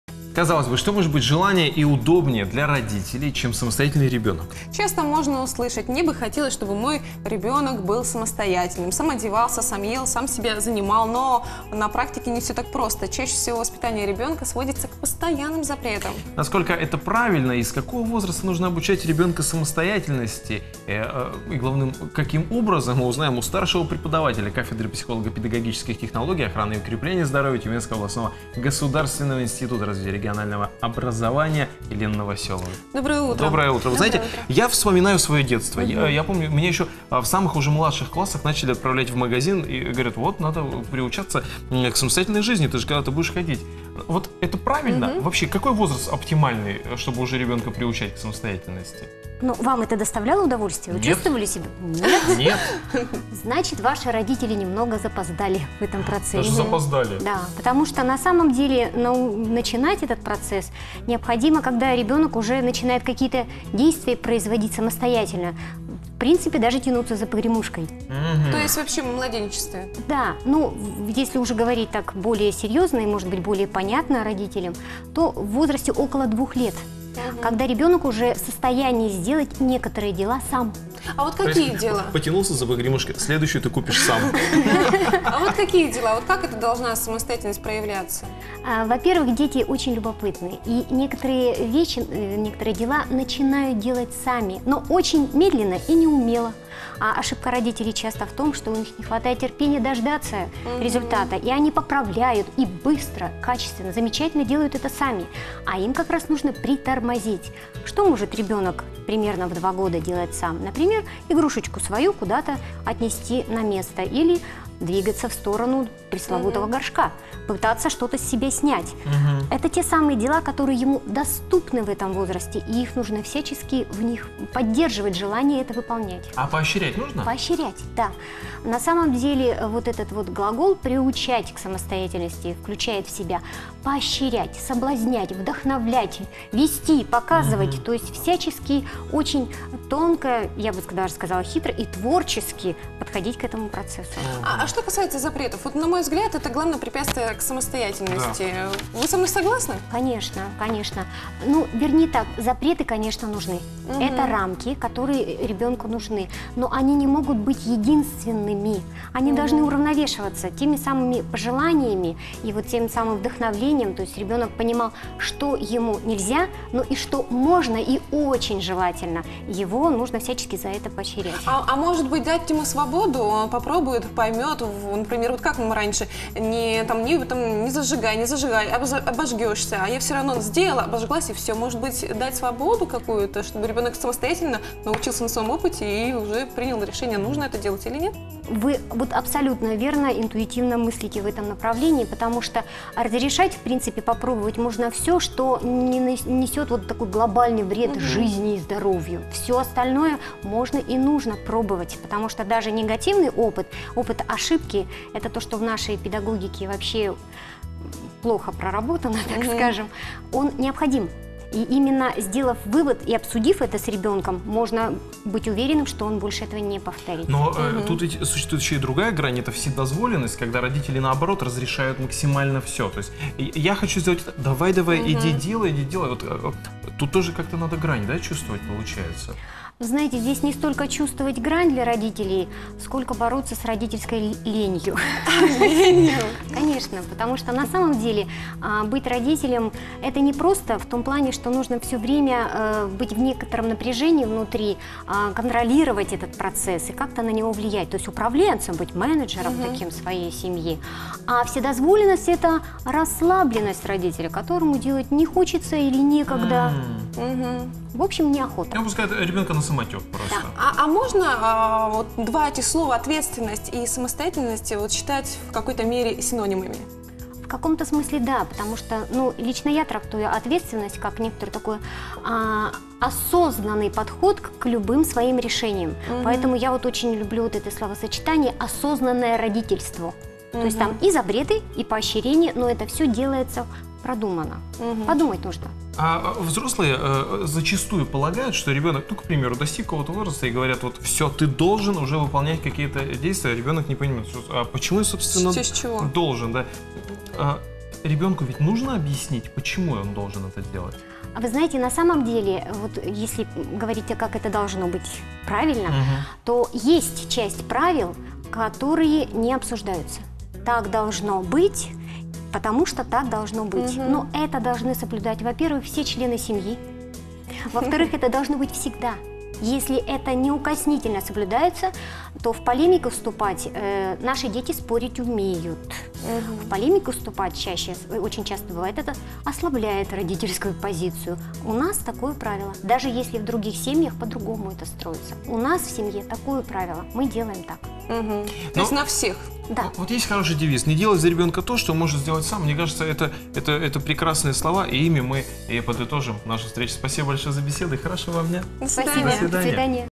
Как научить ребенка самостоятельности
Казалось бы, что может быть желаннее и удобнее для родителя, чем самостоятельный ребенок? Часто можно услышать: "мне бы хотелось, чтобы мой ребенок был более самостоятельным - сам одевался, сам ел, сам себя занимал". Но на практике все не так просто. Чаще всего воспитание ребенка сводится к постоянным запретам. Насколько это правильно и с какого возраста нужно обучать ребенка самостоятельности и главное каким образом - узнаем у старшего преподавателя кафедры психолого-педагогических технологий охраны и укрепления здоровья Тюменского областного государственного института развития регионального образования Елены Новоселовой.